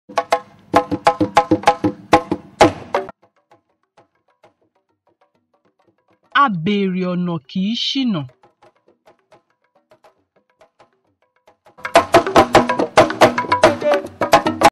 Yorùbá Proverb: "Abèrè ọ̀nà kì í ṣìnà."
Translation: "He who asks for direction will not miss his way."

Lesson: "You have a great potential to be an expert in your chosen field but you need directions from those who have plied that route."